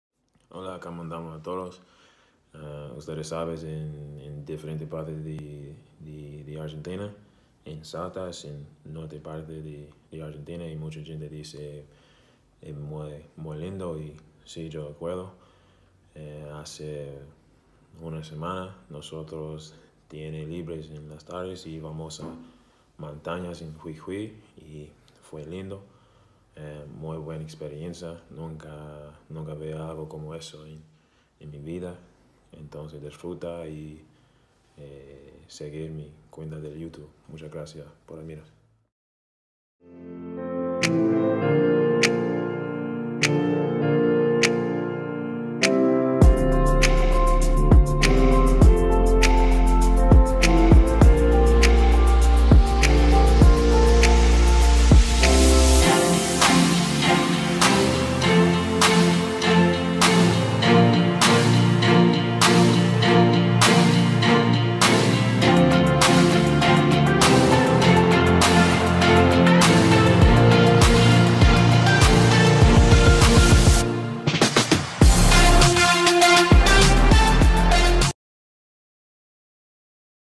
An Argentine excursion to the mountains of Jujuy
Me and my teammates were free after a long week of practice and decided to take a trip to the near by mountains of Jujuy on the outskirts of Salta!